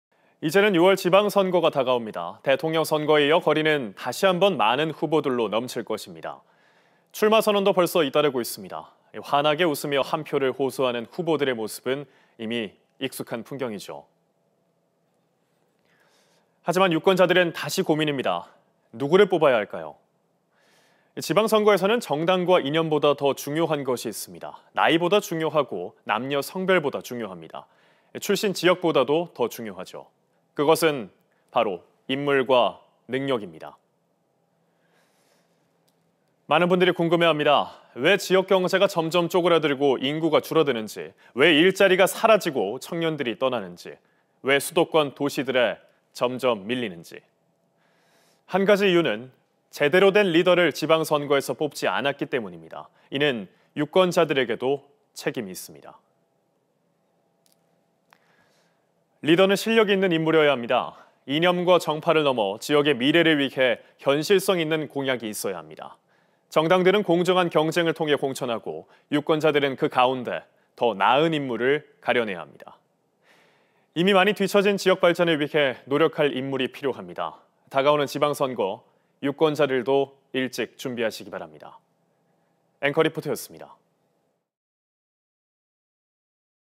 6월 지방선거 대선 뒤 격전 예상
이제는 6월 지방선거가 다가옵니다.

대통령선거에 이어 거리는 다시 한번 많은 후보들로 넘칠 것입니다.

출마 선언도 잇따릅니다.

환하게 웃으며 한 표를 호소하는 후보들의 모습은 이미 익숙한 풍경입니다.

하지만 유권자들은 다시 고민입니다.

누구를 뽑아야 할까요.

지방선거에서는 정당과 이념보다 더 중요한 것이 있습니다.

나이보다 중요하고, 출신 지역보다 중요합니다.

남녀 성별보다도 더 중요합니다.

그것은 바로 인물과 능력입니다.

많은 분들이 궁금해 합니다.

왜 지역경제가 점점 쪼그라들고 인구가 줄어드는지, 왜 일자리가 사라지고 청년들이 떠나는지. 왜 수도권 도시들에 점점 밀리는지.

한 가지 책임은 제대로 된 리더를 지방선거에서 뽑지 않았기 때문입니다.

유권자들에게도 책임이 있습니다.

리더는 실력이 있는 인물이어야 합니다.

이념과 정파를 넘어 지역의 미래를 위해 현실성 있는 공약이 있어야 합니다.

정당들은 공정한 경쟁을 통해 공천하고, 유권자들은 그 가운데 더 나은 인물을 가려내야 합니다.

이미 많이 뒤처진 지역 발전을 위해 노력할 인물이 필요합니다.

다가오는 지방선거, 유권자들도  일찍 준비하시기 바랍니다.

앵커리포트였습니다.